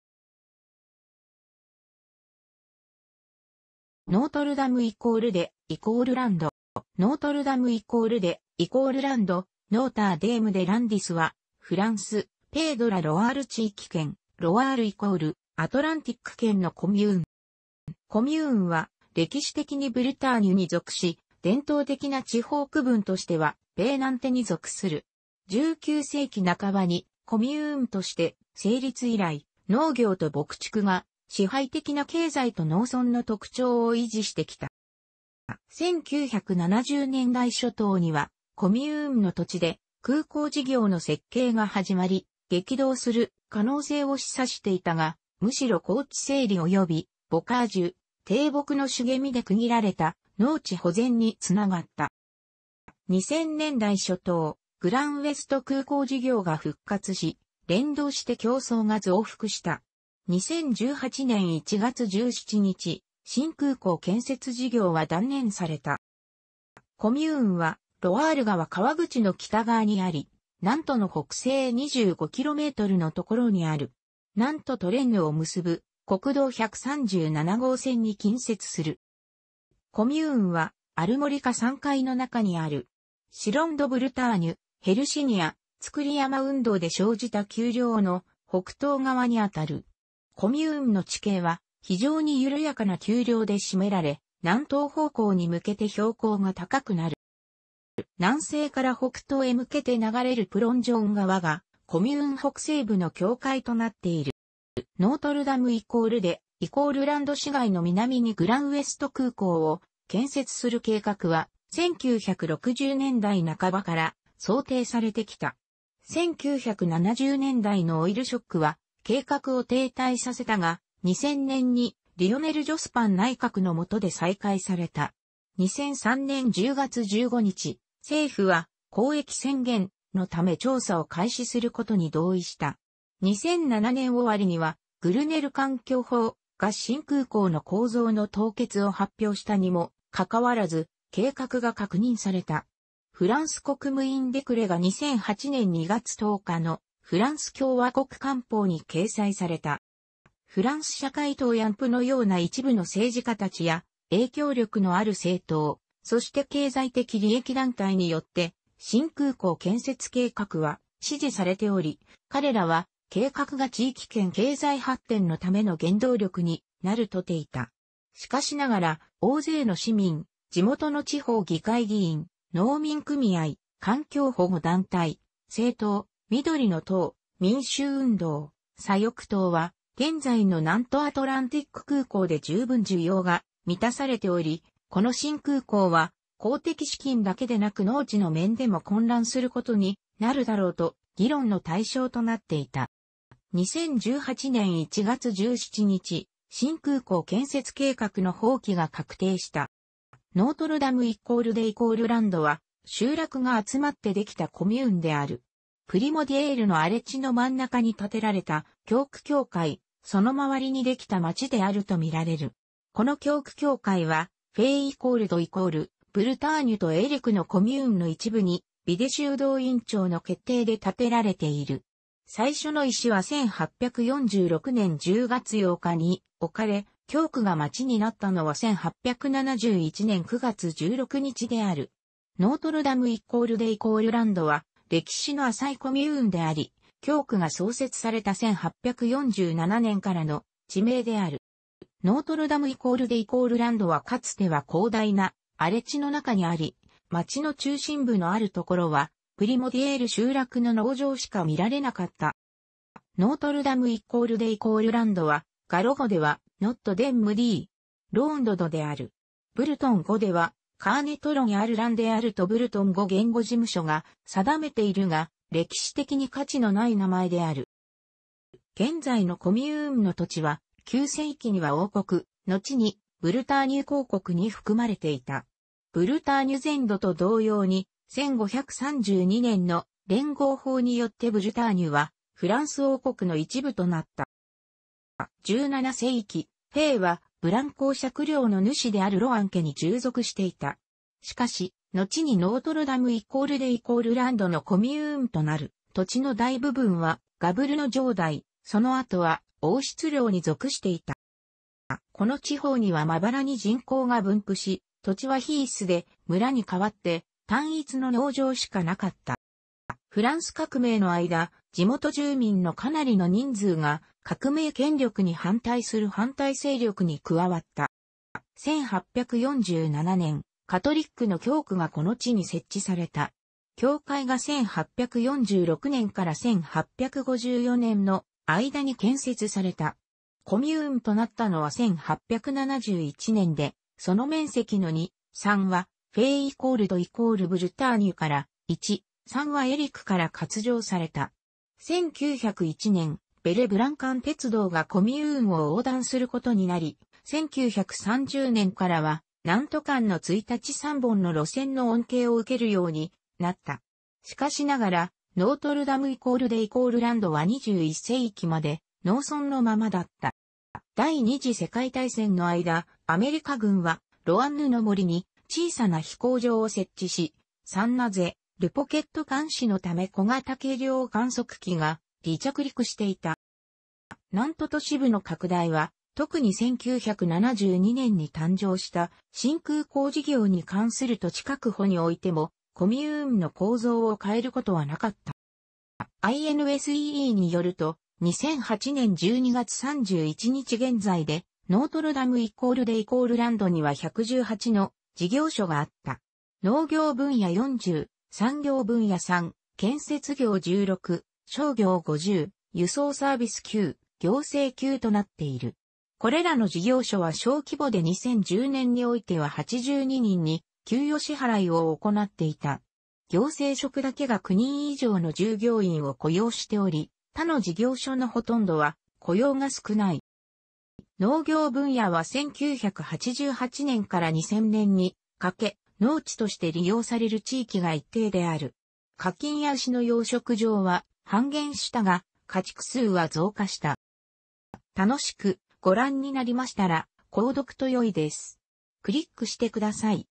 ノートル＝ダム＝デ＝ランド
ノートル＝ダム＝デ＝ランド （Notre-Dame-des-Landes）は、フランス、ペイ・ド・ラ・ロワール地域圏、ロワール＝アトランティック県のコミューン。

コミューンは歴史的にブルターニュに属し、伝統的な地方区分としてはペイ・ナンテに属する。19世紀半ばにコミューンとして成立以来、農業と牧畜が支配的な経済と農村の特徴を維持してきた。

1970年代初頭には、コミューンの土地で空港事業の設計が始まり、激動する可能性を示唆していたが、むしろ耕地整理およびボカージュ（低木の茂みで区切られた農地）保全につながった。

2000年代初頭、グラン・ウェスト空港事業が復活し、連動して競争が増幅した。2018年1月17日、新空港建設事業は断念された。

コミューンはロワール川河口の北側にあり、ナントの北西25kmのところにある。ナントとレンヌを結ぶ国道137号線に近接する。

コミューンはアルモリカ山塊の中にある。シロン・ド・ブルターニュ（ヘルシニア造山運動で生じた丘陵）の北東側にあたる。コミューンの地形は非常に緩やかな丘陵で占められ、南東方向に向けて標高が高くなる。

南西から北東へ向けて流れるプロンジョン川が、コミューン北西部の境界となっている。

ノートル＝ダム＝デ＝ランド市街の南にグラン・ウエスト空港を建設する計画は、1960年代半ばから想定されてきた。1970年代のオイル・ショックは計画を停滞させたが、2000年にリオネル・ジョスパン内閣のもとで再開された。2003年10月15日、政府は公益宣言（）のため調査を開始することに同意した。2007年終わりには、グルネル環境法（）が新空港の構造の凍結を発表したにもかかわらず、計画が確認された。フランス国務院デクレが2008年2月10日のフランス共和国官報に掲載された。

フランス社会党やUMPのような一部の政治家たちや影響力のある政党、そして経済的利益団体によって、新空港建設計画は支持されており、彼らは計画が地域圏経済発展のための原動力になるとみていた。しかしながら、大勢の市民、地元の地方議会議員、農民組合、環境保護団体、政党（緑の党、民主運動、、左翼党）は、現在のナント・アトランティック空港で十分需要が満たされており、この新空港は公的資金だけでなく農地の面でも混乱することになるだろうと、議論の対象となっていた。

2018年1月17日、新空港建設計画の放棄が確定した。

ノートル＝ダム＝デ＝ランドは集落が集まってできたコミューンである。プリモディエールの荒地の真ん中に建てられた教区教会、その周りにできた町であると見られる。この教区教会は、フェイ＝ド＝ブルターニュとエリクのコミューンの一部に、ビデ修道院長の決定で建てられている。最初の石は1846年10月8日に置かれ、教区が町になったのは1871年9月16日である。ノートル＝ダム＝デ＝ランドは歴史の浅いコミューンであり、教区が創設された1847年からの地名である。

ノートル＝ダム＝デ＝ランドはかつては広大な荒地の中にあり、町の中心部のあるところは、プリモディエール集落の農場しか見られなかった。

ノートル＝ダム＝デ＝ランドは、ガロ語では"Not·rdinm dé. Laundd"である。ブルトン語では"Kernitron-al-Lann"であるとブルトン語言語事務所が定めているが、歴史的に価値のない名前である。

現在のコミューンの土地は、9世紀には王国（のちにブルターニュ公国）に含まれていた。ブルターニュ全土と同様に、1532年の連合法によってブルターニュはフランス王国の一部となった。

17世紀、フェイはブラン侯爵領の主であるロアン家に従属していた。しかし、のちにノートル＝ダム＝デ＝ランドのコミューンとなる土地の大部分は、ガヴルの城代、その後は王室領に属していた。

この地方にはまばらに人口が分布し、土地はヒースで、村に代わって単一の農場しかなかった。

フランス革命の間、地元住民のかなりの人数が革命権力に反対する反対勢力に加わった。

1847年、カトリックの教区がこの地に設置された。教会が1846年から1854年の間に建設された。コミューンとなったのは1871年で、その面積の2/3はフェイ＝ド＝ブルターニュから、1/3はエリクから割譲された。1901年、ベレ-ブラン間鉄道がコミューンを横断することになり、1930年からはナント間の一日3本の路線の恩恵を受けるようになった。しかしながら、ノートル＝ダム＝デ＝ランドは21世紀...